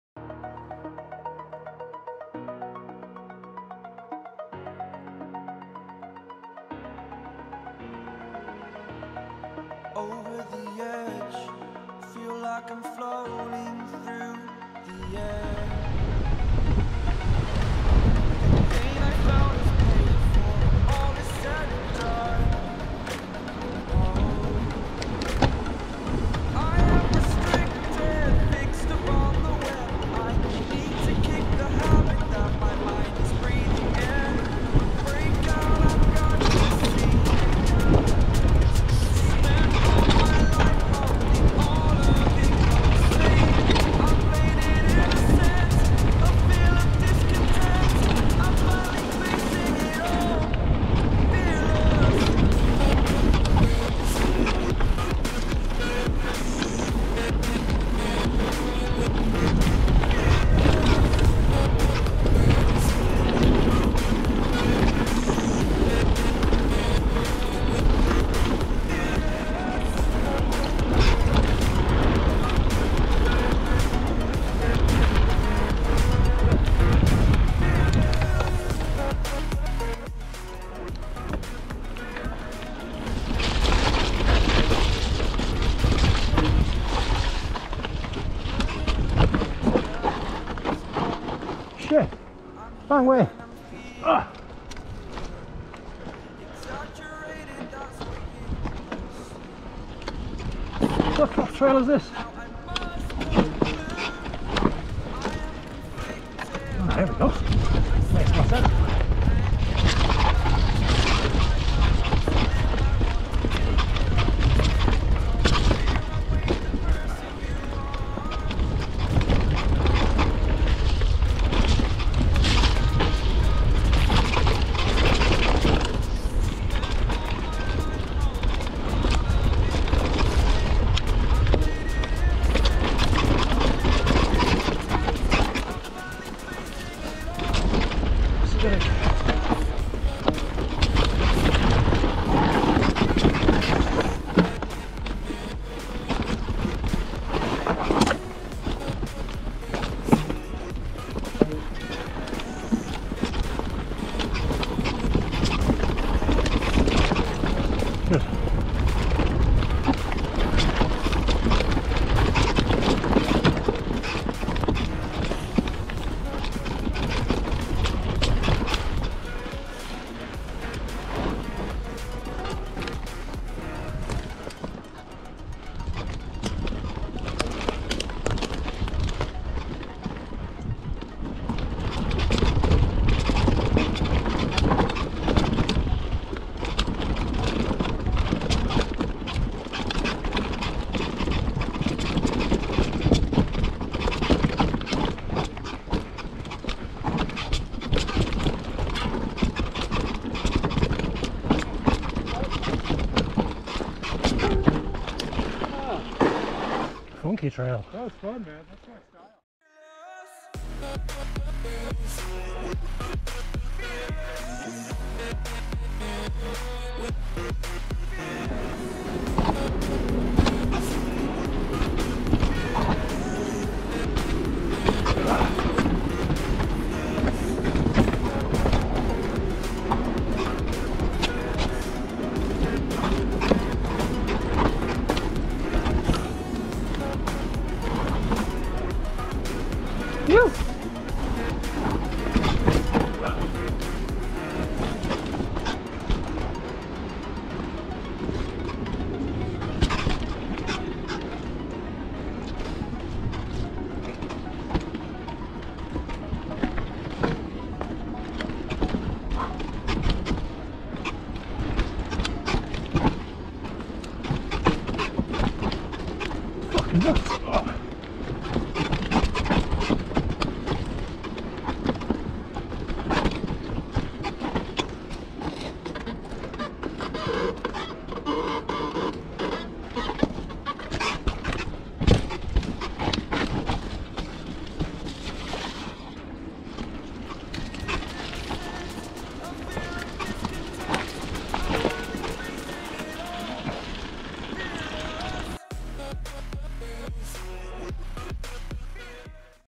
Unmarked Trails Ticknock
Bike;
The Stereo 120 Race 29 - Full Suspension.

Camera;
Go pro 9 Black with max lens mod

Location;
Ticknock MTB trails